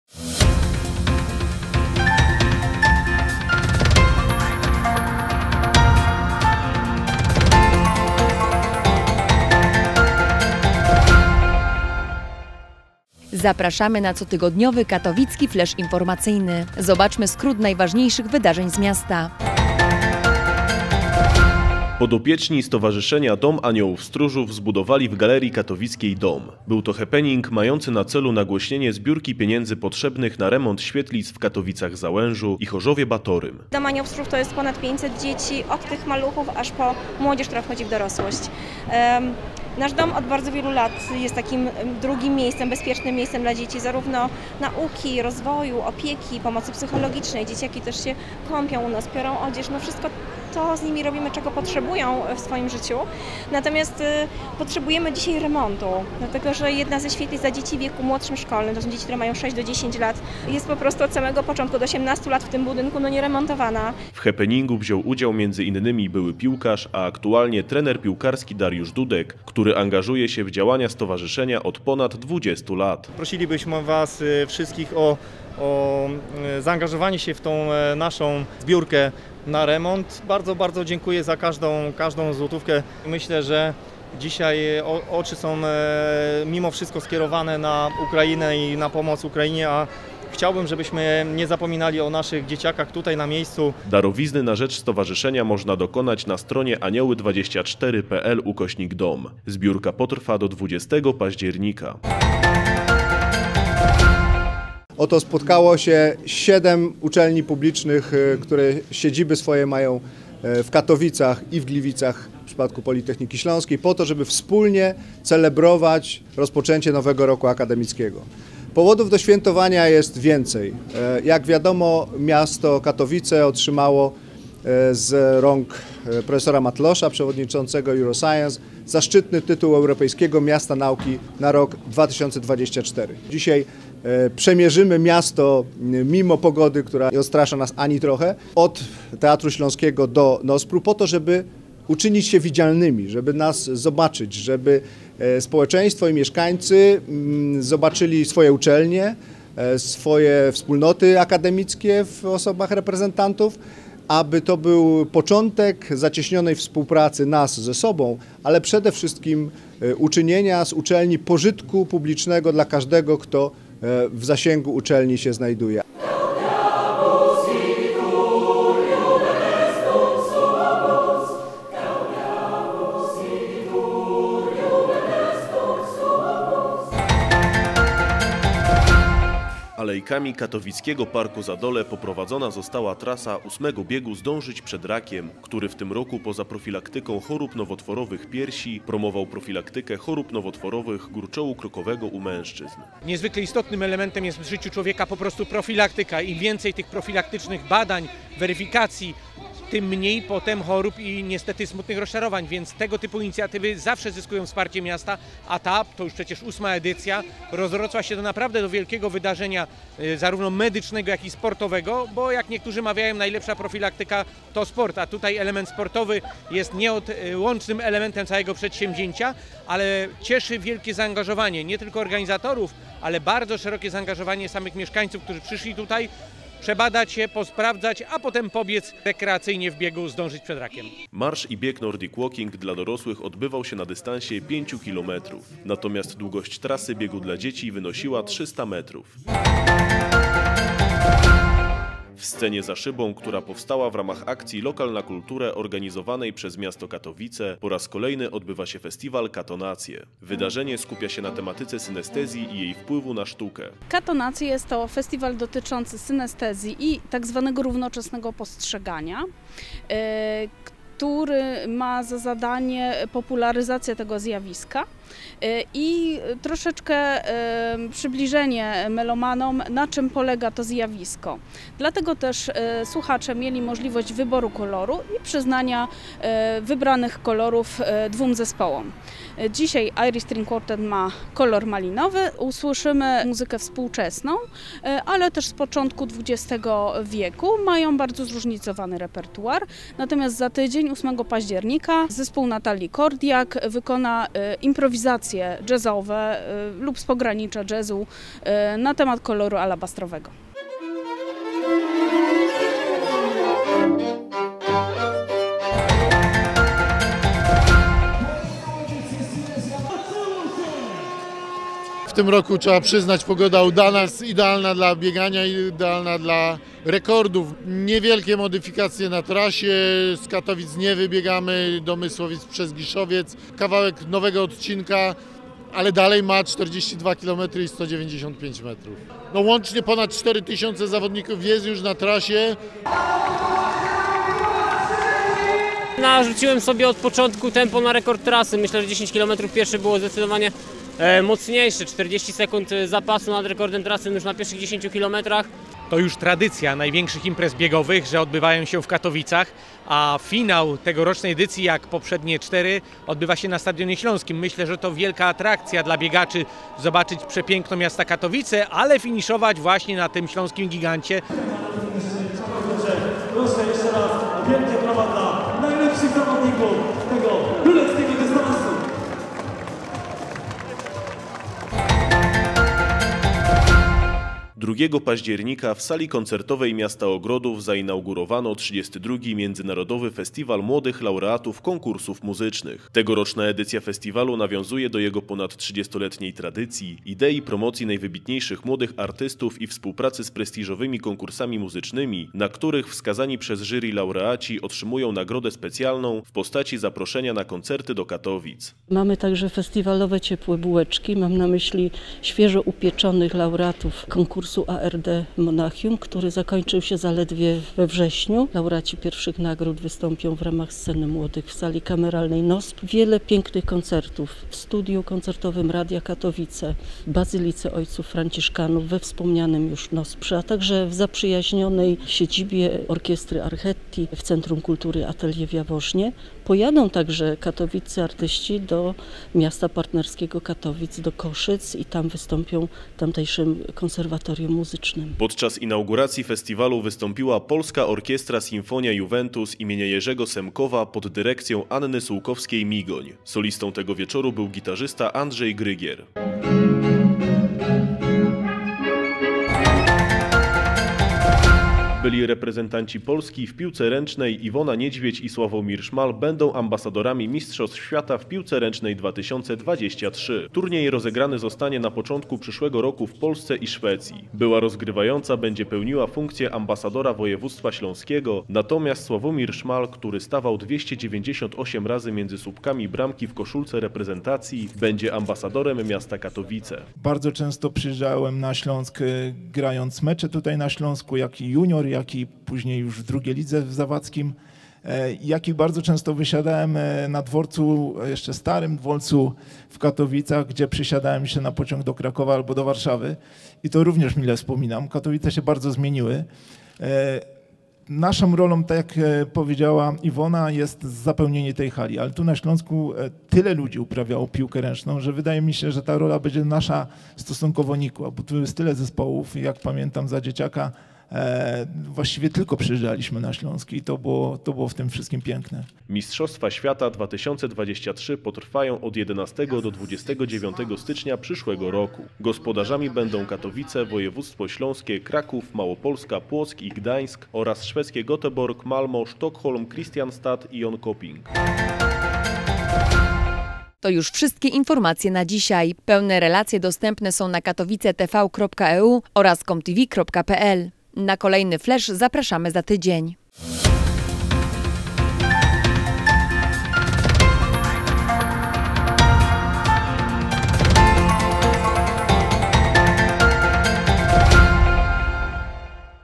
KATOWICKI FLESZ INFORMACYJNY 4.10.2022
W tym wydaniu:
- Cegiełka na rzecz Domu Aniołów Stróżów
- Międzyuczelniania Inaugracja Roku Akademickiego
- Bieg "Zdążyć przed rakiem"
- Katonacje
- Silesia Marathon
- 32. Międzynarodowy Festiwal Młodych Laureatów Konkursów Muzycznych
- 100 dni do Mistrzostw Świata w piłce ręcznej